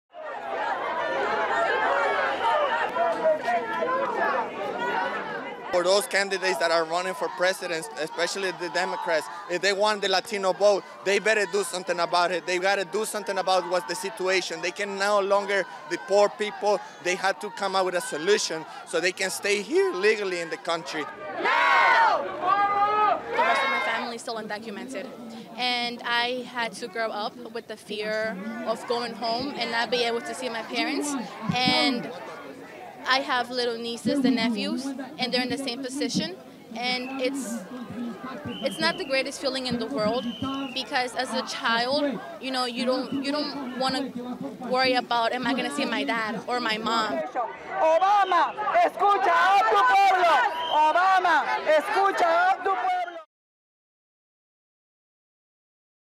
Protesters Hold Anti-Deportation Demo In Front Of White House
December 30, 2015 - Scores of people gathered in front of the White House on Wednesday to protest the Obama administration's plan to deport Central American immigrant families starting in January 2016. Protesters said while many of these immigrants are illegal, deporting them would separate families and could leave children abandoned.